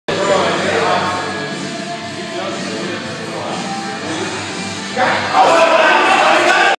Nathan Slaughter 43" Vertical
2014 NFL Draft prospect performs vertical jump.